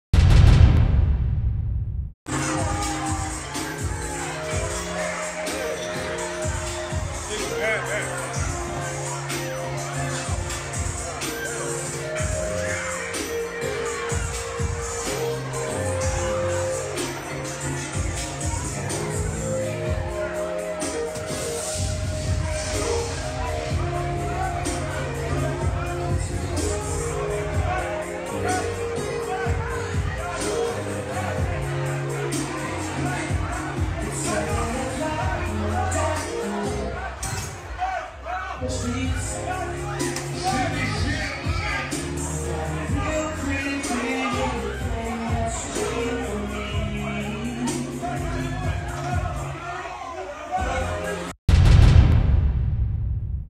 'L&HH' Stars Arrested in Atlanta, Wild Video Shows Brawl | TMZ
Some of 'Love & Hip Hop's stars got busted in Atlanta last night -- and based on this video of the brawl ... they should consider a name change, 'cause there was no love here at all.